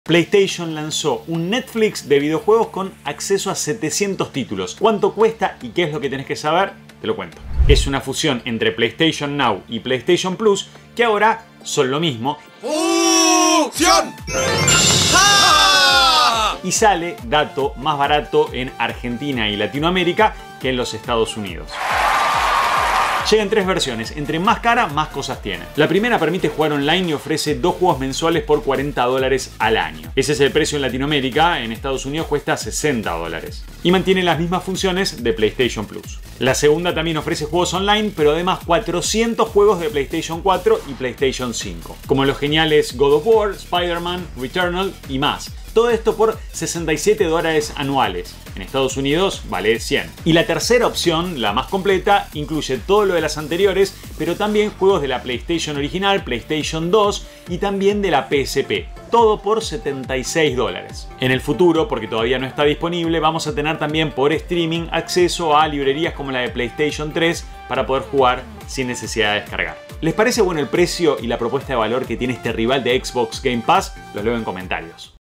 NOVEDADES en PLAYSTATION
Te cuento las claves sobre el "Netflix" de videojuegos de Playstation. Sus precios en pesos al escribir esta nota serán:
PlayStation Plus Essential 7.640 pesos
PlayStation Plus Extra 12.800 pesos
PlayStation Plus Deluxe 14.700
¿Qué les parece?